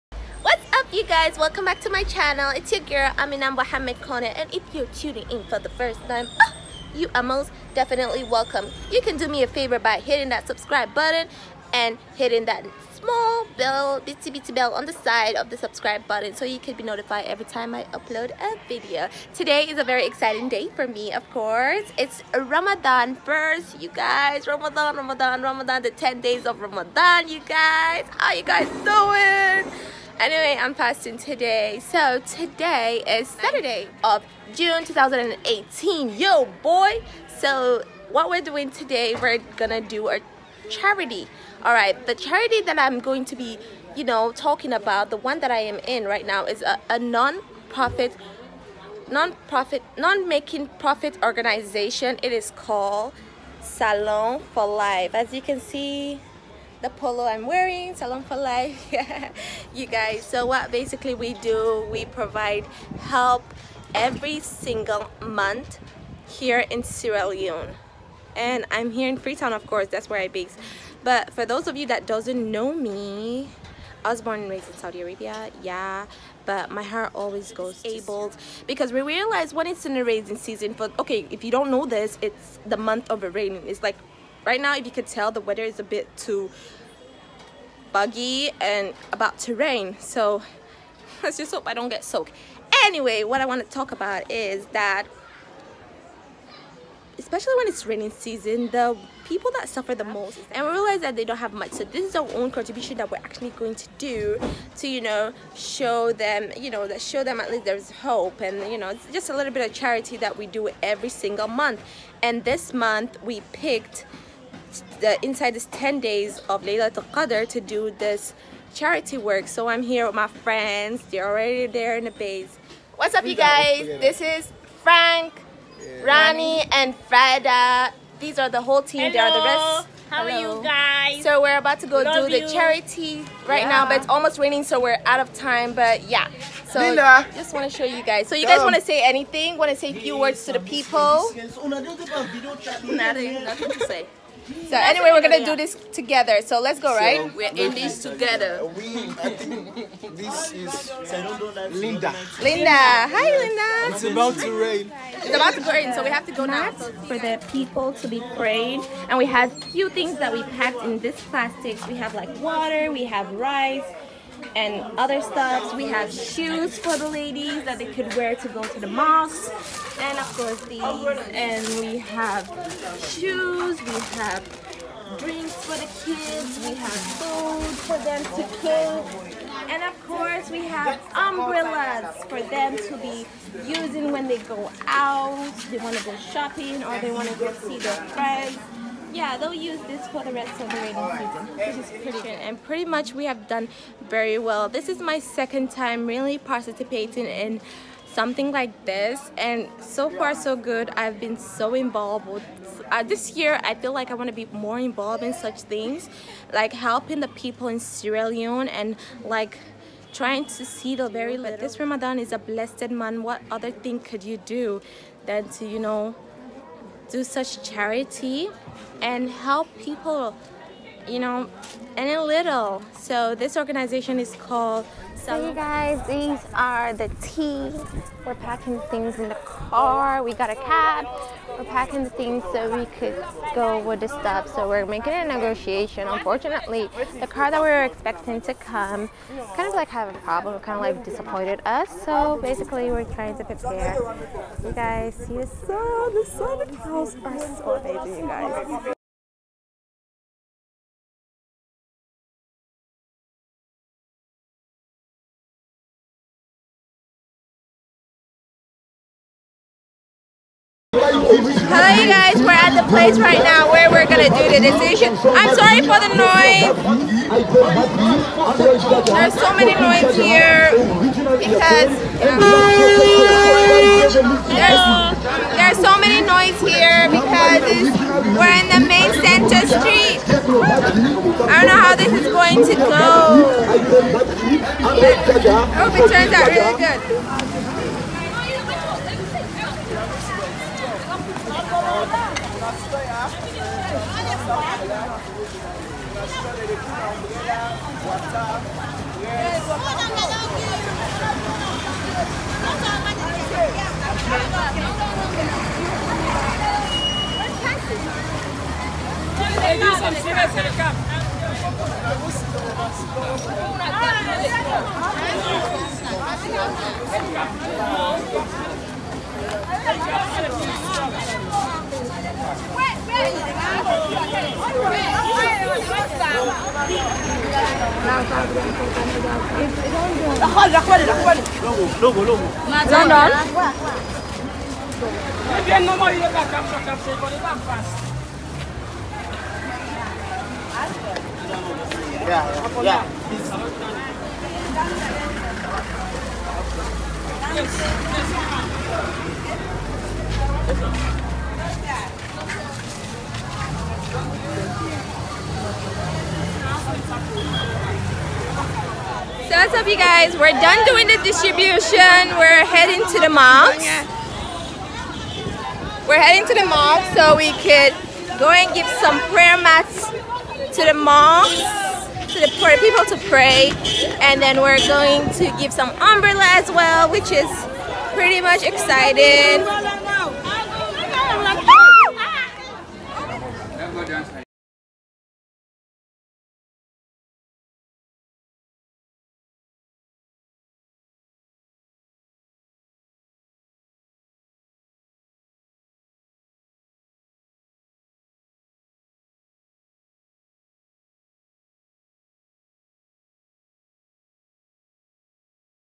Ramadan Vlog / Sierra Leone 4 life! Charity❤️ 👭
it’s Ramadan and I’m with an organization called Sierra Leone for life🇸🇱🇸🇱 we’re doing a charitable work which I’m pretty much excited to share with you guys.

And we are planning on to build an orphanage home here in Sierra Leone, freetown so please donate  and  help the people and children for a better,beautiful future🇸🇱🇸🇱.. In Sha Allah

Sierra Leone 4 life contacts

Facebook: Sierra Leone for life
Instagram: Sierra Leone for life

Facebook: RiRi M Corneh
Instagram: RiRi M Corneh

XOXO ✌🏾